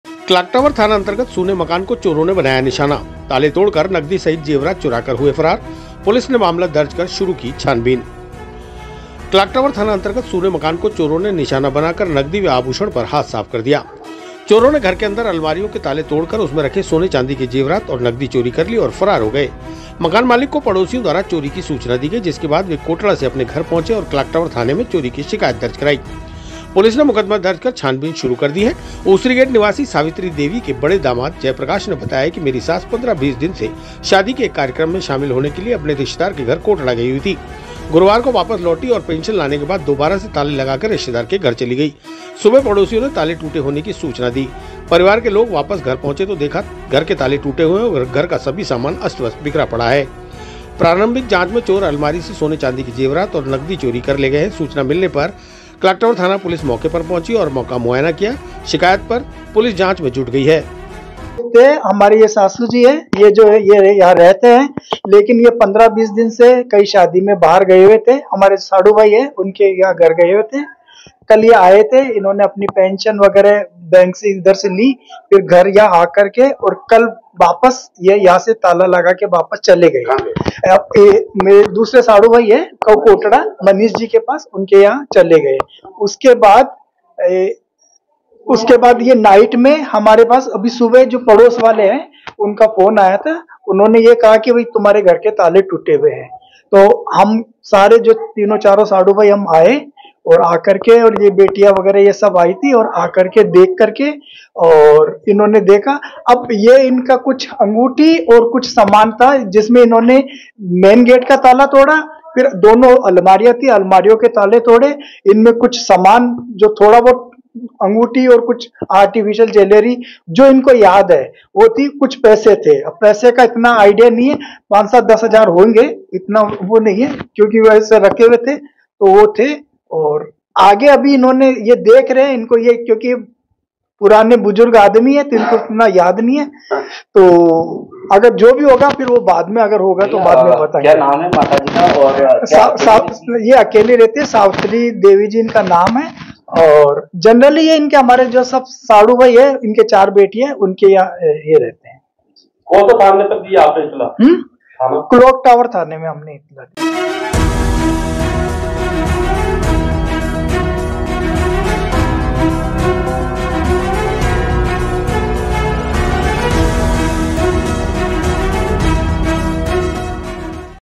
क्लॉक टावर थाना अंतर्गत सुने मकान को चोरों ने बनाया निशाना,
क्लॉक टावर थाना अंतर्गत सुने मकान को चोरों ने बनाया निशाना, ताले तोड़कर नगदी सहित जेवरात चुरा कर हुए फरार,
पुलिस ने मामला दर्ज कर शुरू की छानबीन